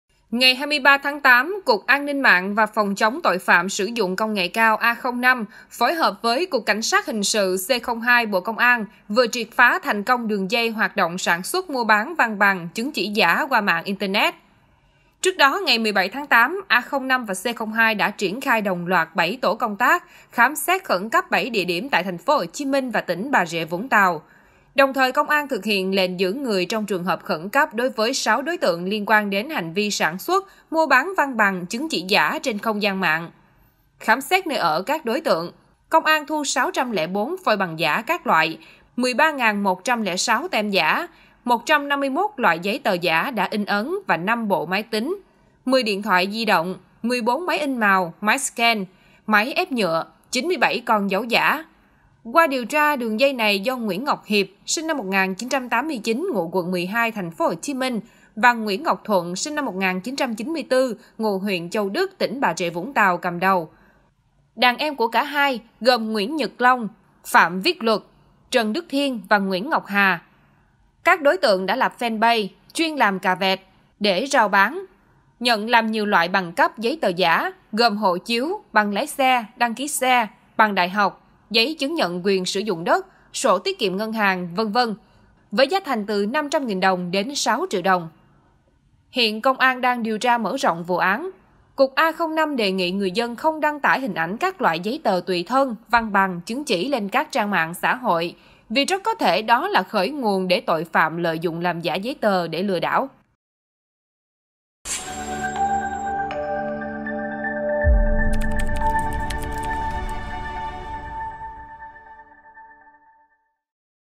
Triệt phá đường dây sản xuất, mua bán văn bằng, chứng chỉ giả qua mạng cực khủng
Ngày 23-8, Cục An ninh mạng và phòng chống tội phạm sử dụng công nghệ cao (A05) phối hợp với Cục Cảnh sát hình sự (C02), Bộ Công an vừa triệt phá thành công đường dây hoạt động sản xuất, mua bán văn bằng, chứng chỉ giả qua mạng Internet.